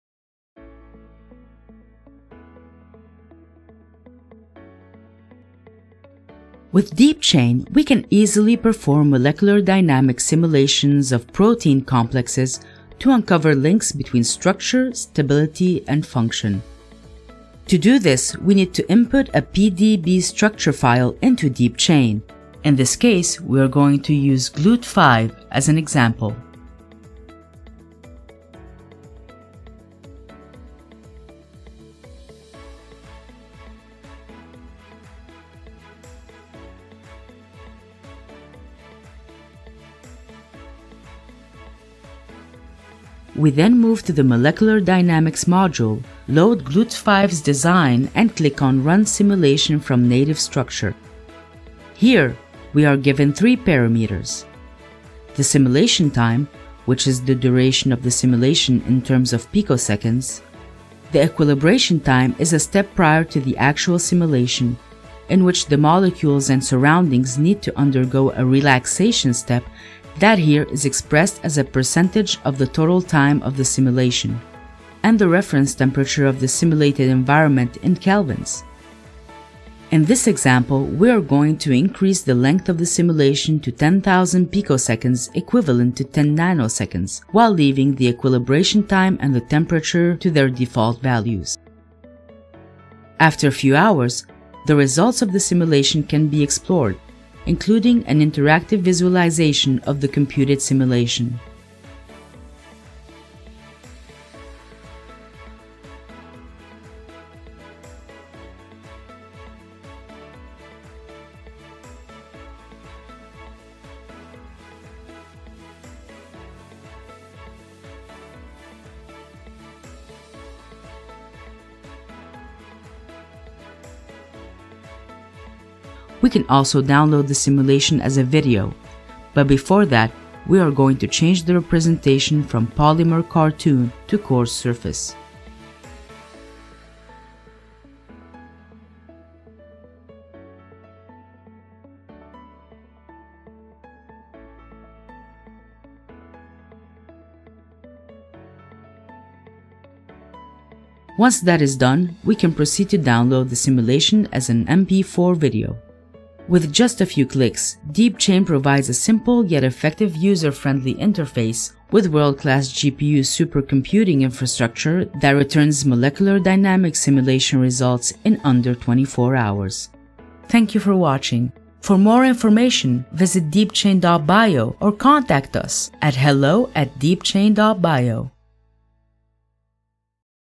Facilitating powerful molecular dynamics simulations to accelerate protein research
Molecular Dynamics Simulations provide a wealth of information to uncover links between protein structure, stability and function. Generally, running these simulations requires substantial expertise and compute resources. DeepChain™ has been launched as a user-friendly AI-powered protein design platform featuring a molecular dynamics simulation module to increase access to such a powerful research tool to all scientists.